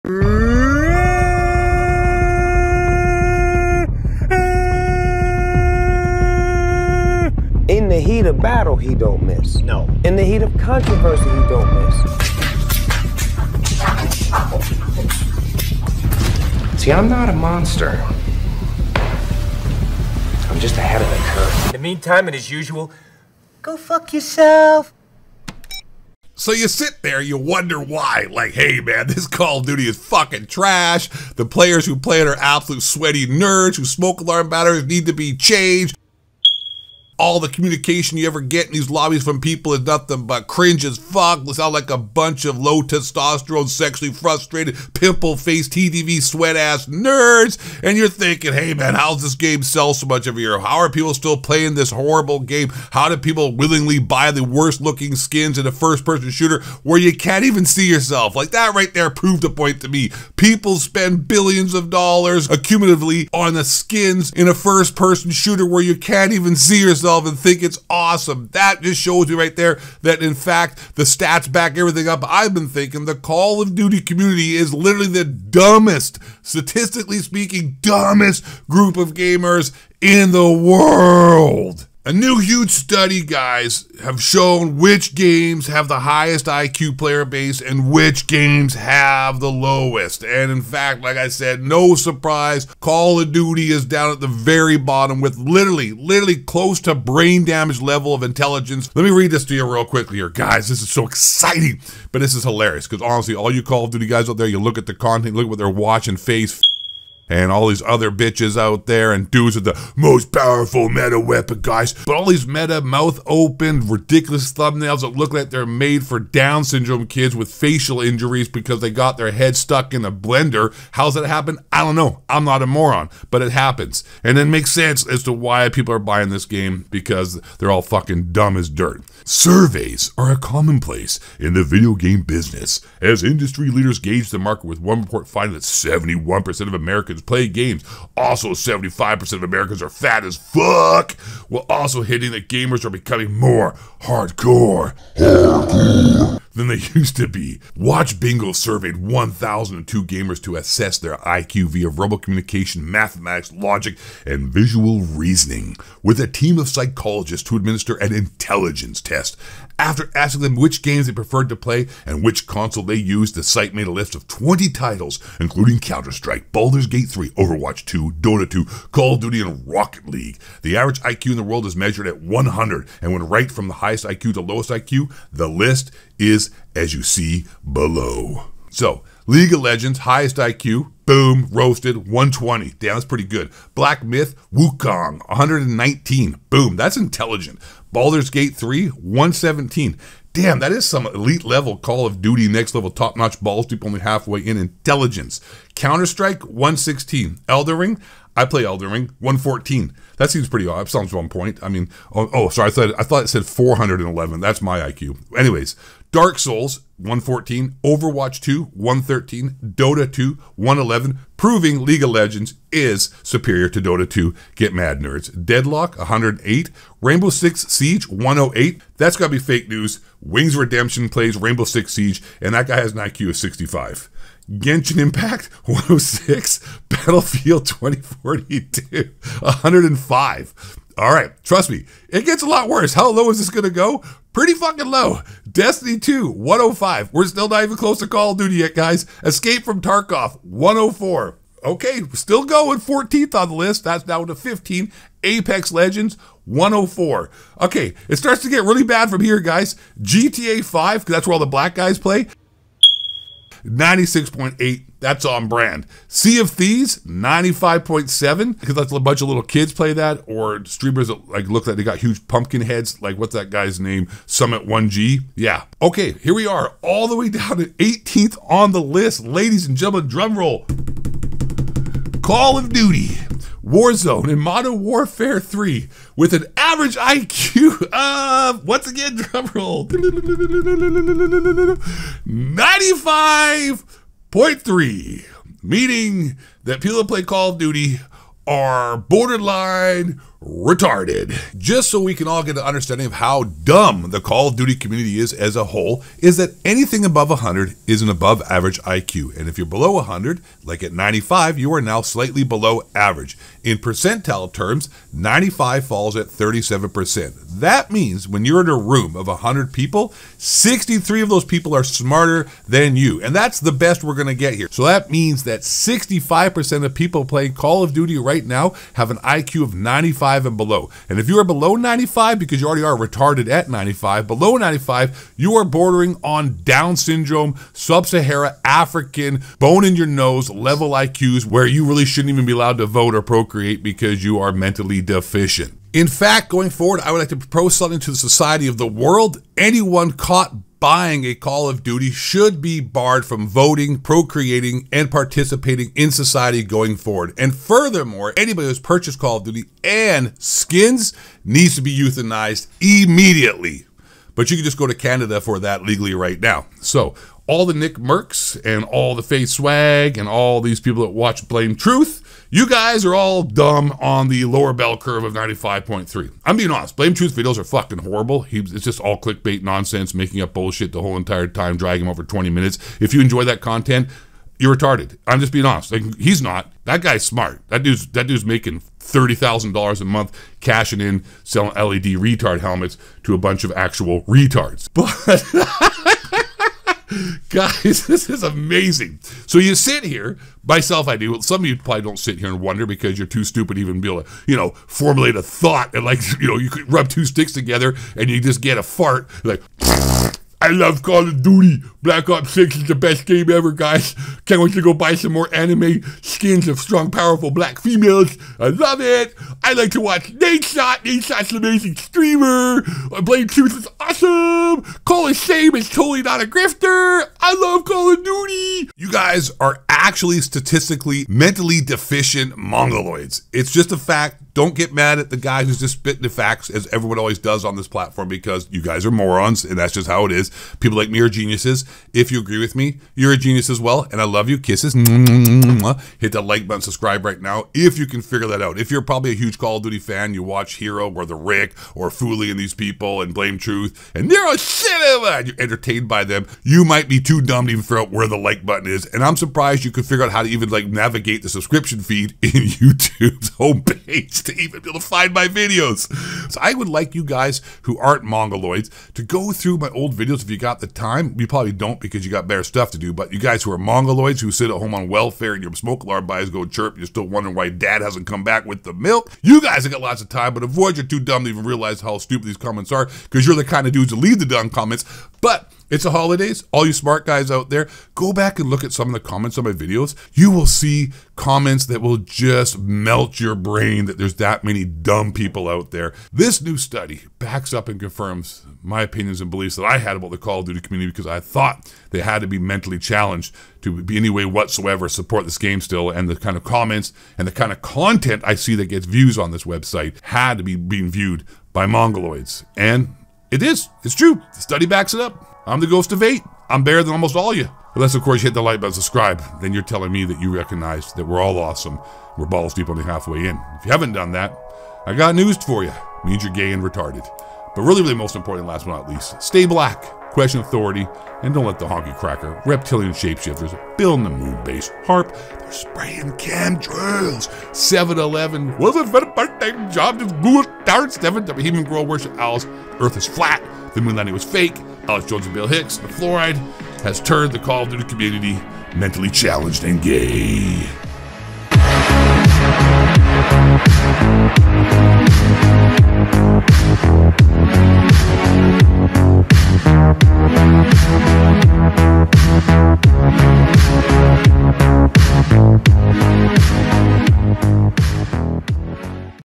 STUDY PROVES CALL OF DUTY PLAYERS LITERALLY RET*RDED
New study came out showing IQ's of certain gaming communities. No surprise, Call of Duty was at the very bottom with an average IQ of 95. Let's see who's the smartest gamers in the world since Call of Duty gamers are the dumbest.